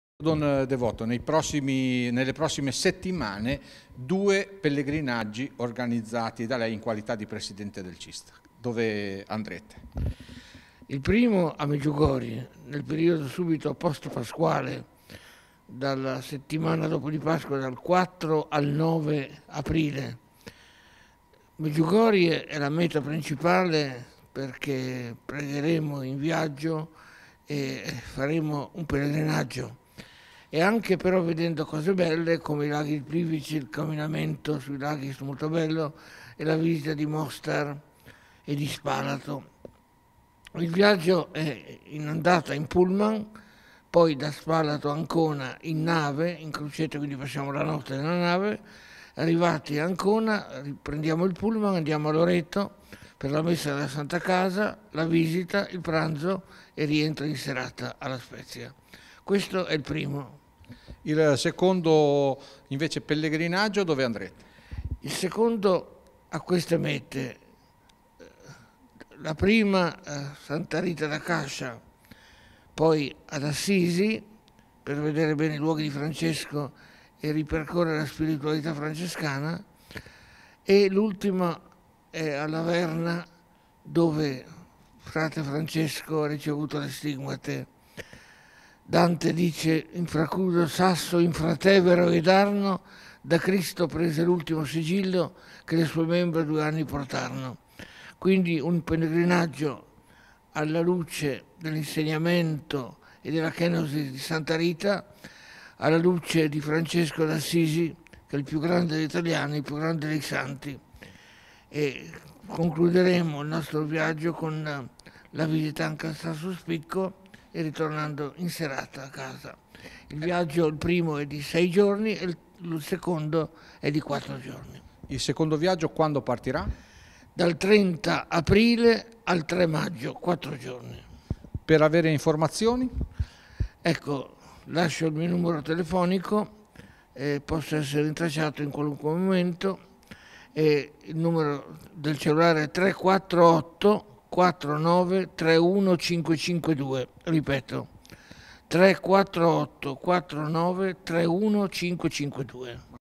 CITS, pellegrinaggi a Medjugorje e Santa Rita 26-02-2018
Don Devoto, presidente Centro Italiano per il Turismo Sociale, ha presentato i pellegrinaggi in due importanti simboli della fede cattolica: Medjugorie e i luoghi francescani.